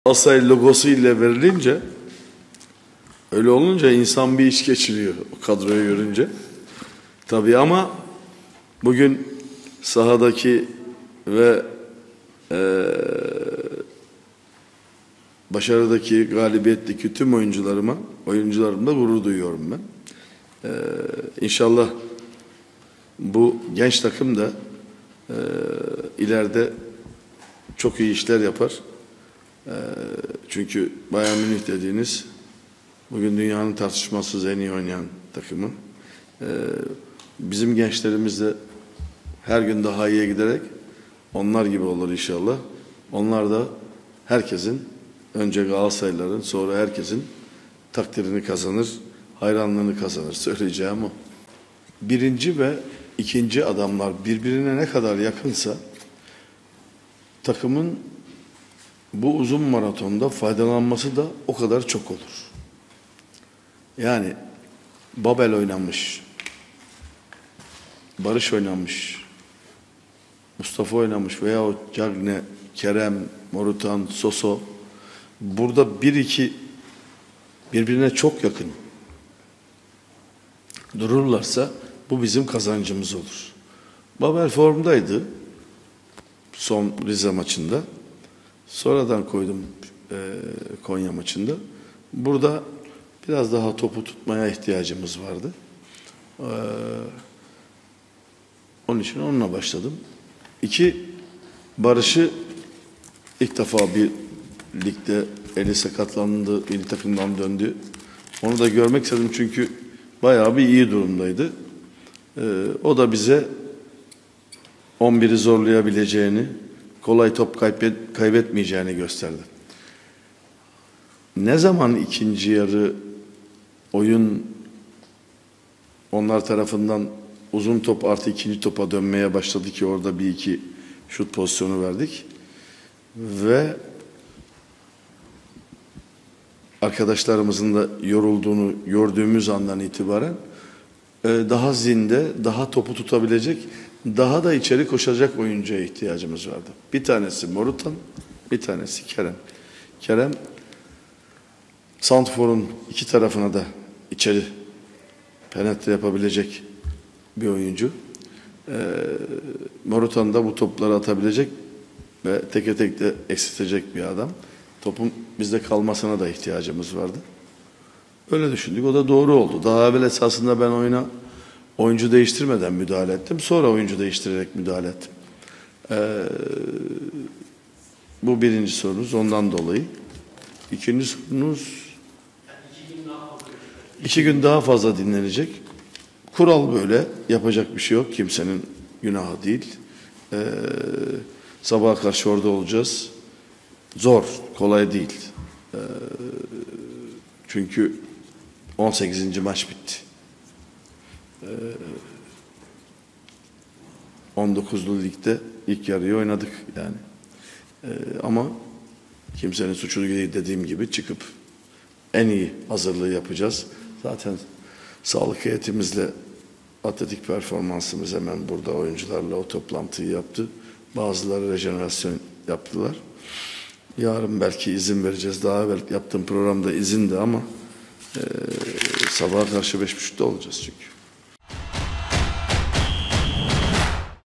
Fatih Terim'i Bayern Münih teknik direktörü yaptılar | "İnsan o kadroyu görünce bir iç geçiriyor"
UEFA Avrupa Ligi'nde Galatasaray, Lokomotiv Moskova'ya konuk oldu. Mücadele öncesi stadyumdaki ekranda sarı-kırmızılıların kadrosu yerine Bayern Münih'in 11'i yazıldı.